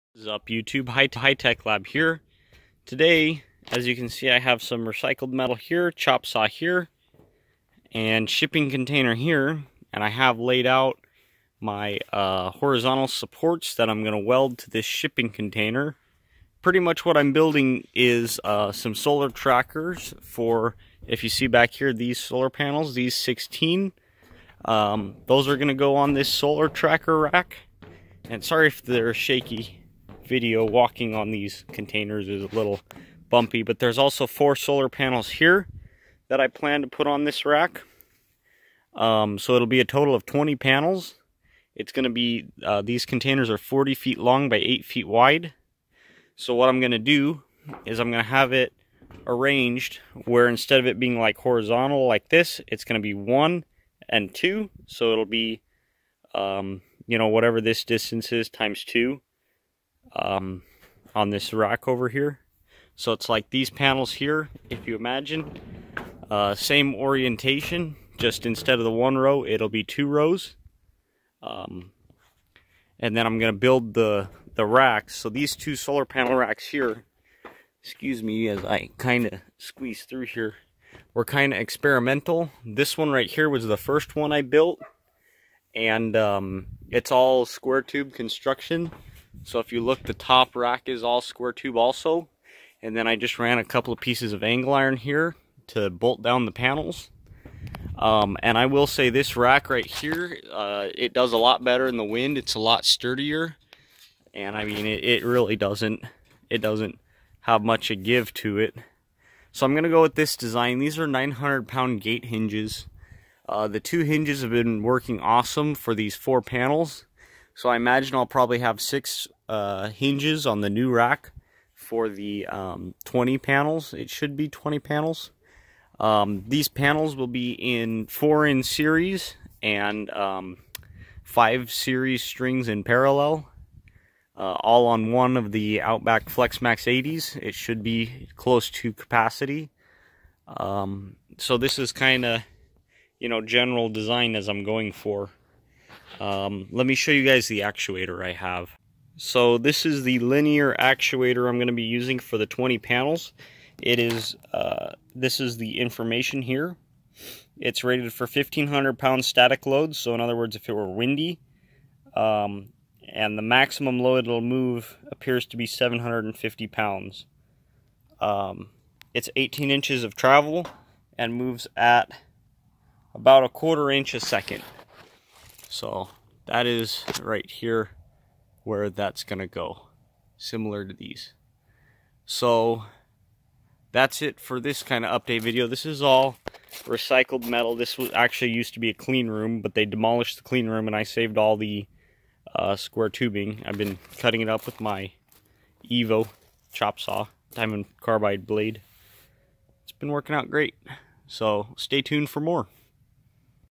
Off Grid Solar Tracker Build Pt 1
In this video series, I will show the process of building a solar panel tracker that will hold 20x 210 watt solar panels, totaling approximately 4200 watts of solar power.